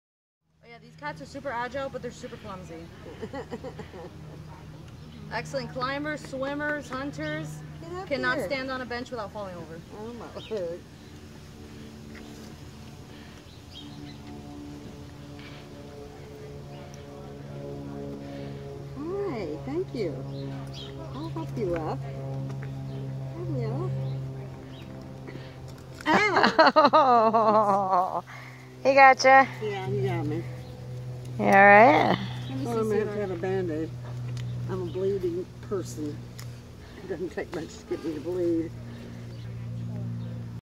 Vicious jaguar attack....
It was an accident.  He didn't mean to bite her arm.   He was just going for the toy.